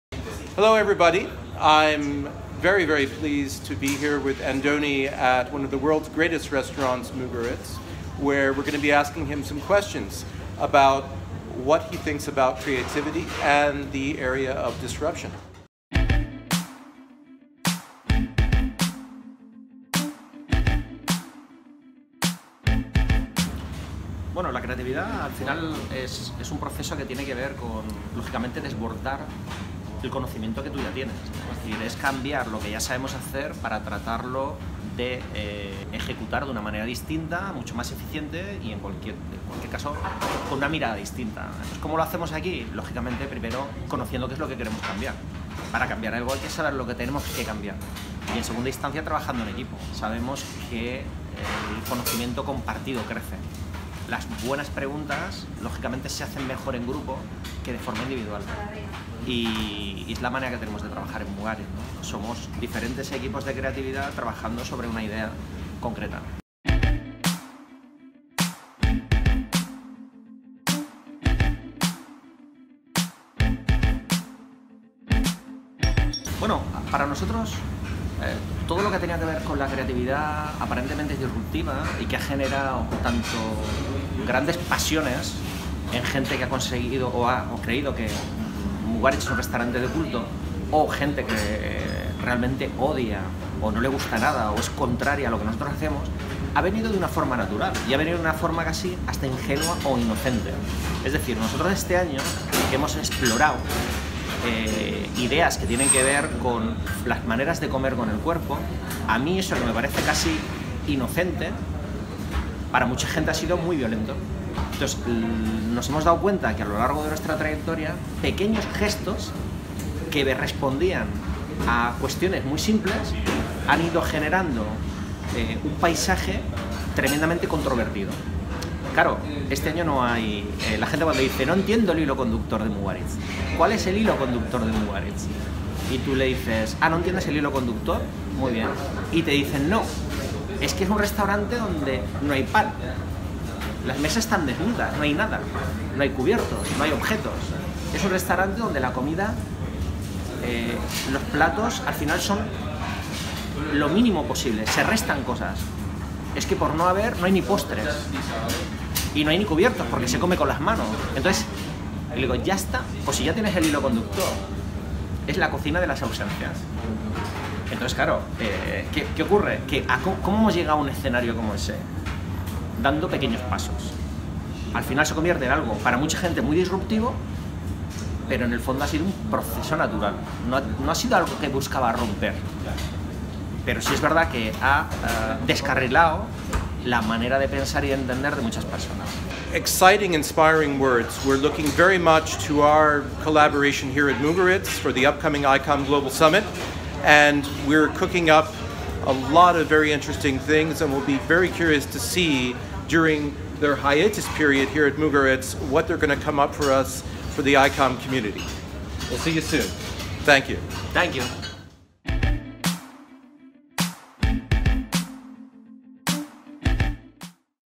Chef Andoni talks about upcoming I-COM '18 Dinner at Mugaritz
The I-COM Global Summit 2018 will close later than usual with a special treat this year. Thursday (Apr 12th) finishes with end post-dinner at midnight with an exclusive dinner at restaurant Mugaritz.

Mugaritz currently enjoys the position of No. 9 on TheWorlds50Best list. "Andoni Luis Aduriz, simply known as Andoni, is considered by many observers to be the natural heir to the title of Spain’s most pioneering chef after Ferran Adrià from Restaurant El Bulli".
Coinciding with the 20th anniversary of Mugaritz, Andoni will prepare a special menu showcasing the evolution of their cuisine throughout the years. In 2010, a fire in the restaurant proved to be an important disruptor, as afterwards the type of cuisine shifted dramatically in favour of innovation.

In this video, I-COM Chairman, Andreas Cohen interviews Chef Andoni about his views on Culinary Creativity and Disruption.

The I-COM Global Summit is the world's leading event on Smart Data Marketing, where the who's who from Marketing Data & Measurement gather from around the world to inspire, connect and develop.